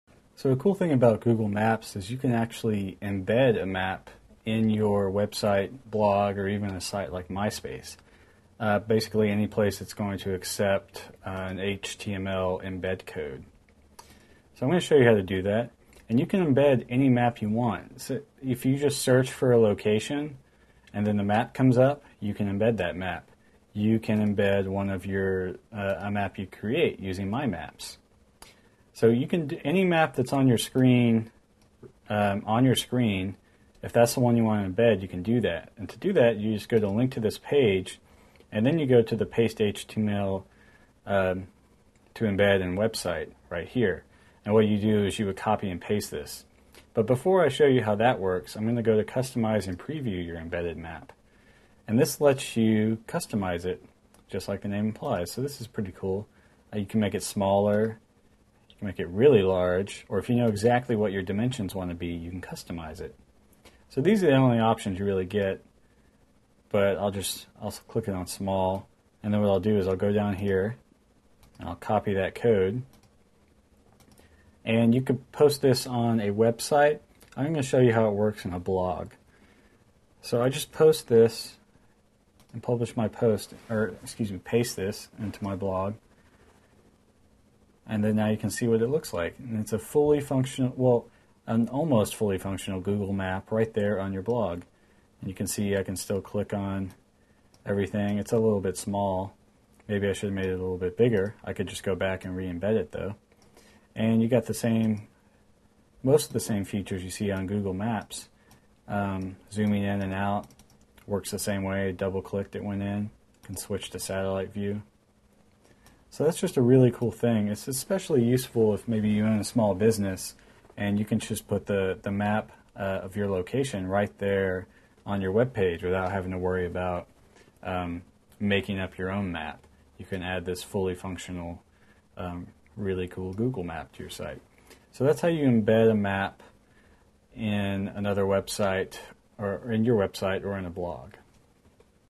Adding Google Maps to Blogs or Emails
Adding Google Maps to Blogs or Emails. Part of the series: How to Use Google Maps. How to add Google Maps to blogs and emails; learn more about Google features and interfaces in this free instructional video.